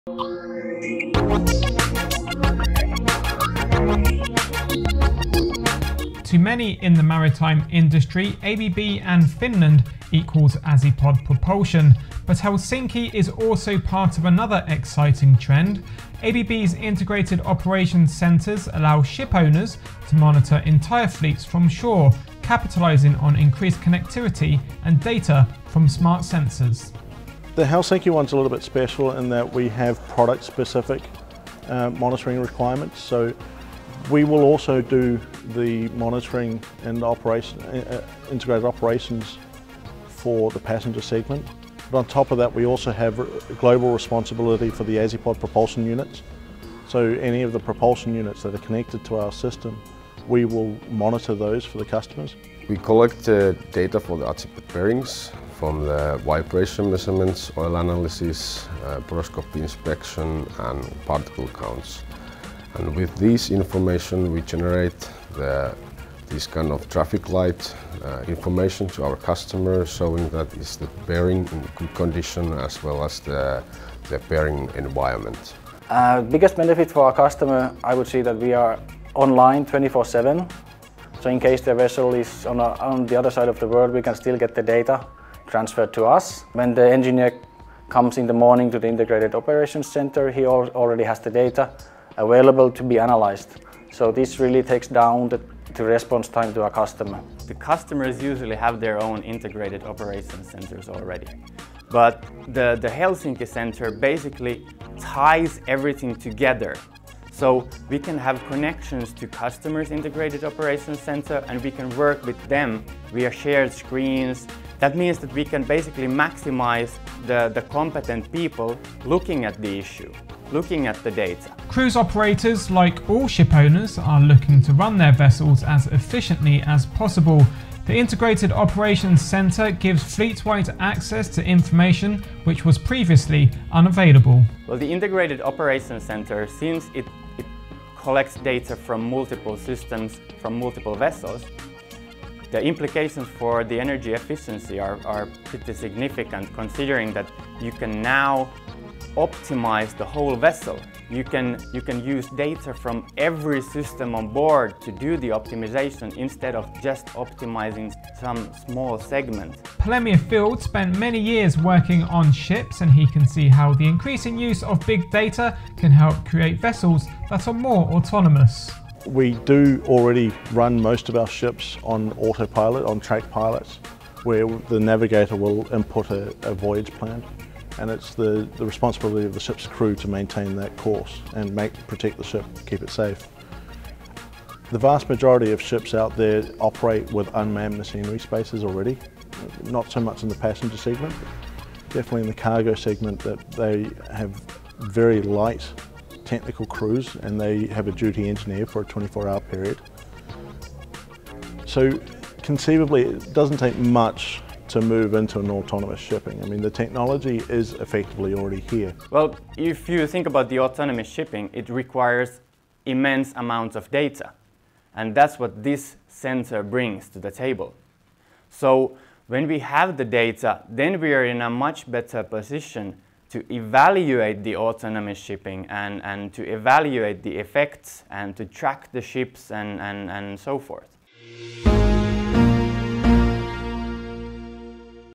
ABB opens Helsinki Integrated Operations Center
ABB is rolling out its Integrated Operations concept across the globe. Here, the brains behind the Helsinki Center discuss how it will target Azipod propulsion and the passenger segment in particular.